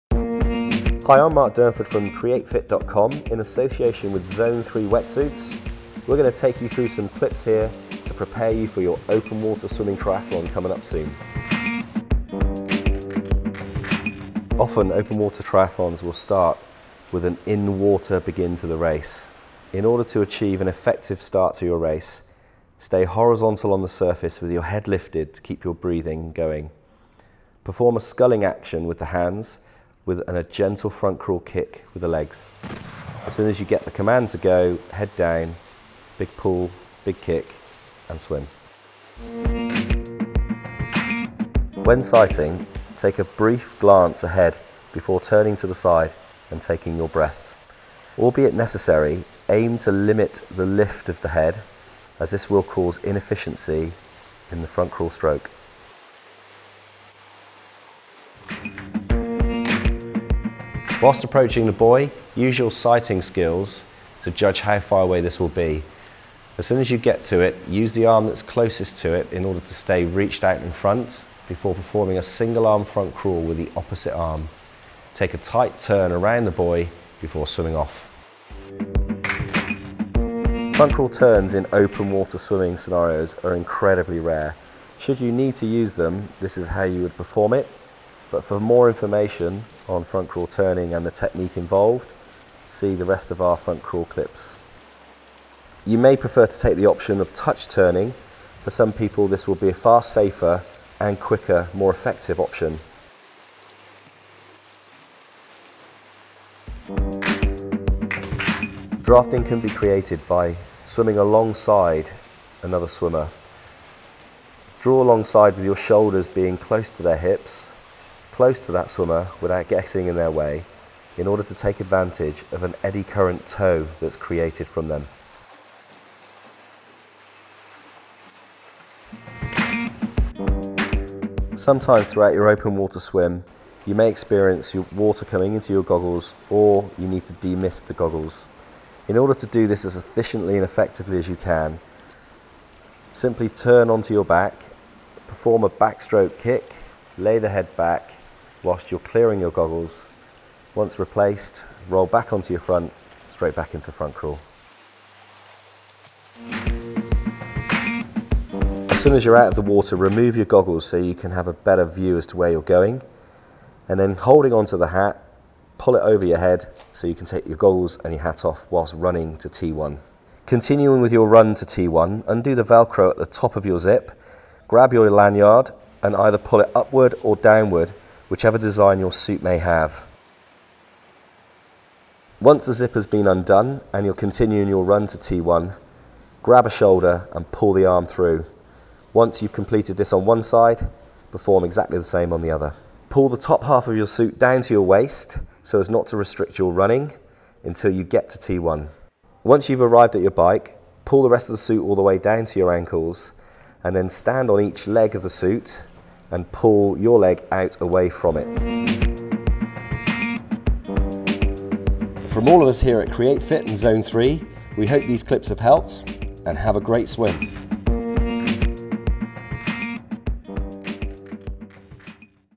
How to prepare for Triathlon Swimming
A compilation of clips demonstrating all the skills necessary to help you ensure an effective and enjoyable open water triathlon swim.  In association with Zone3 wetsuits.